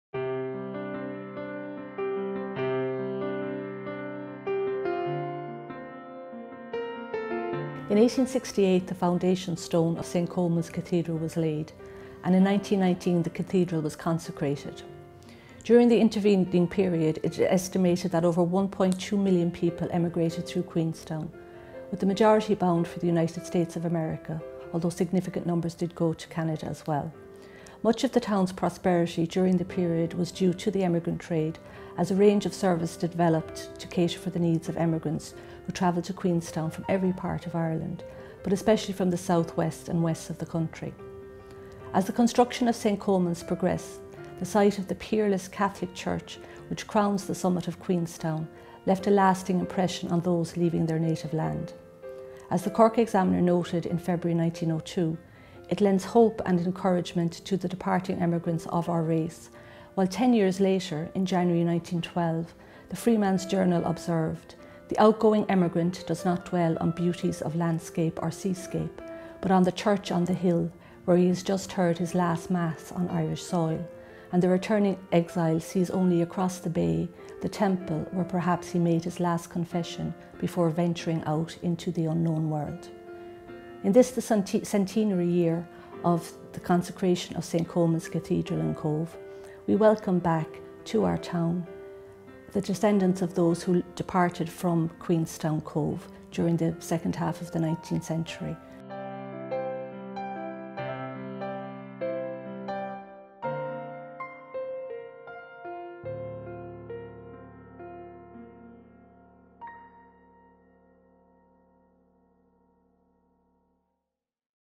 Cathedral's Construction - Marita Foster
This StColmansCentenary video sees historian Marita Foster describe the period of the Cathedrals construction which also witnessed 1.2 million emigrants departing from Cobh/Queenstown. Many celebrated their last mass at the Cathedral before setting sail to the USA, Canada & Australia